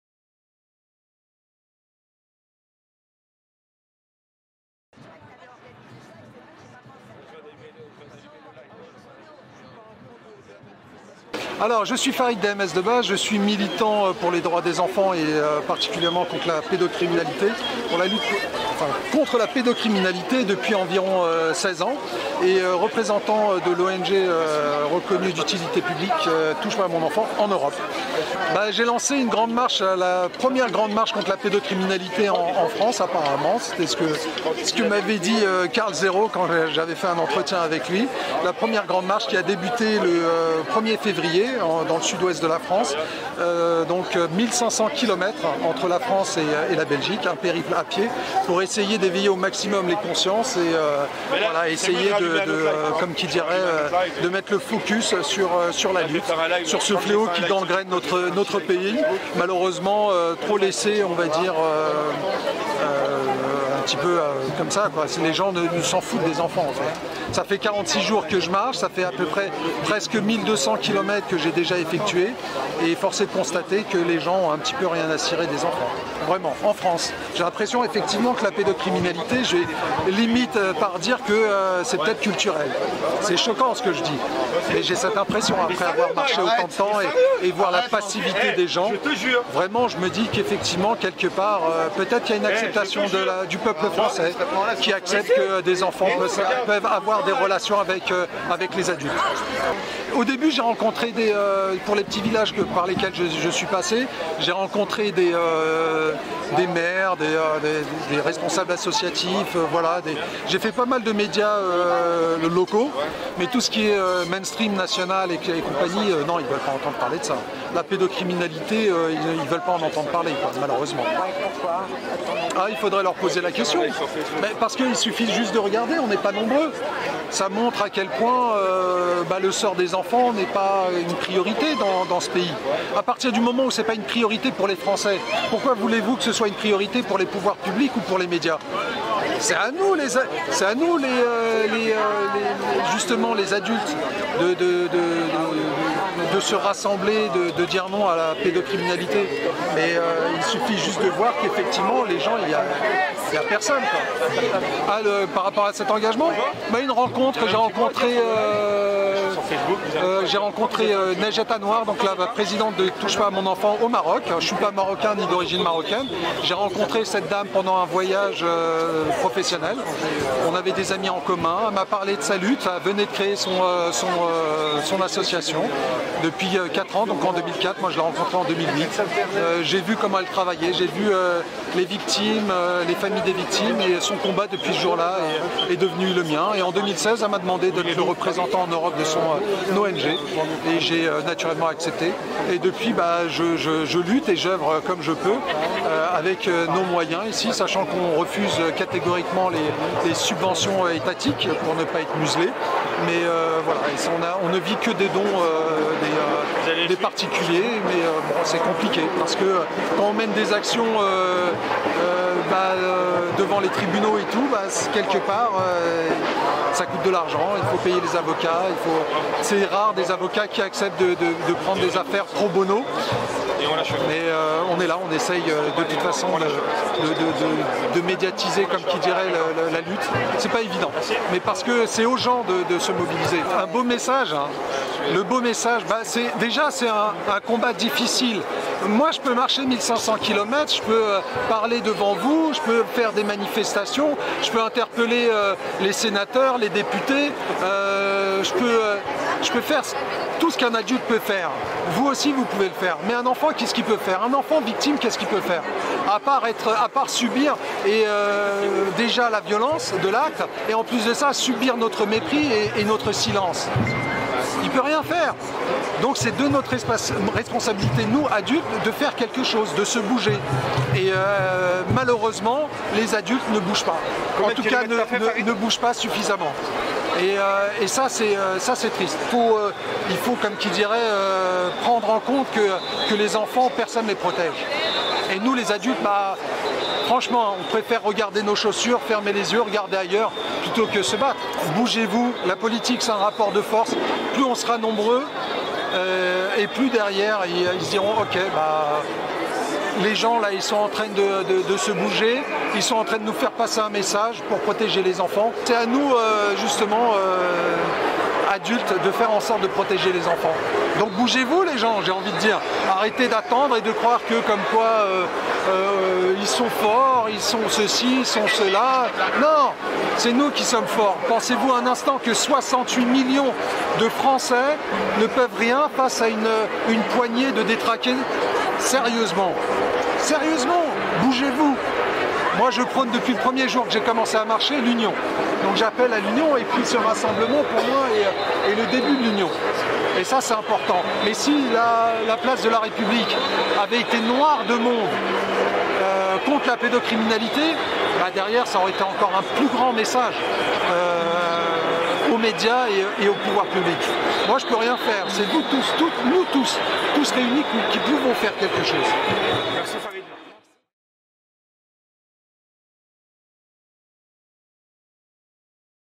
La Grande Marche contre la pédocriminalité
Farid Dms Debah a entamé une Grande Marche contre la Pédocriminalité à travers toute la France. Il est parti d'Albi pour atteindre Bruxelles pour déposer une pétition afin d'interdire les sites et/ou applications de rencontres pour mineurs infiltrés par des pédocriminels. Pressenza était présent pour son arrivée à Paris ce 23 mars 2024.